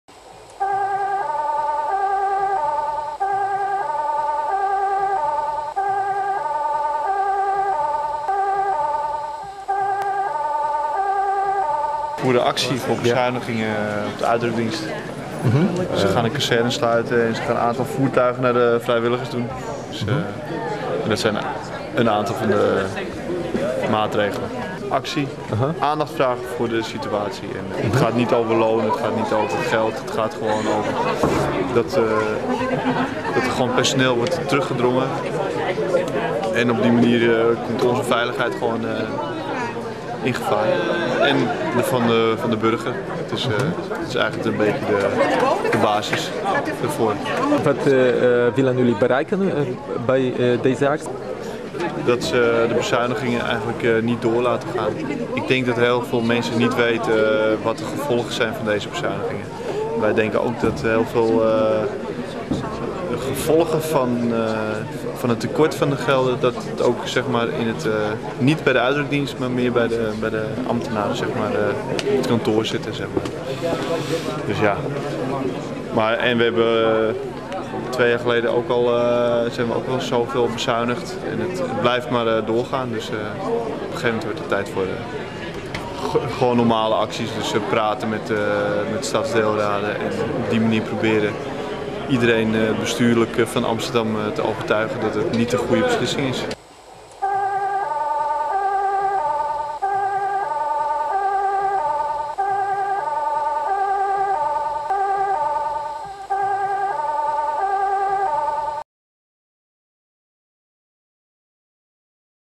Brandweer Tegen Bezuinigingen.
Beste Amsterdammers,De brandweer Amsterdam-Amstelland en vakbond Abvakabo FNV voeren actie voor de veiligheid van alle inwoners van Amsterdam. De gemeente Amsterdam wil fors bezuinigen op het brandweerkorps.De gemeente wil 3,5 miljoen euro bezuinigen, bovenop de bezuiniging van 4 miljoen euro die de brandweer de afgelopen jaren al heeft bezuinigd. Dat betekent onder andere dat de kazerne in Amsterdam-Zuidoost moet sluiten, dat minder ladderwagens beschikbaar zijn en dat er minder mensen op een autospuitwagen zitten. Daarnaast moeten vrijwilligers meer taken overnemen van de beroepsbrandweer.

Deze maatregelen kosten arbeidsplaatsen op plekken waar ze hard nodig zijn. Daardoor moet de brandweer in noodsituaties de broodnodige ondersteuning missen en is het korps minder goed voorbereid. Dat gaat mensenlevens kosten!

Doe mee aan de e-mailactie hieronder en laat de gemeente Amsterdam weten wat je vindt van de bezuinigingen.

Namens alle leden van het brandweerkorps Amsterdam-Amstelland, vakbond Abvakabo FNV en de inwoners van Amsterdam: hartelijk dank voor uw steun!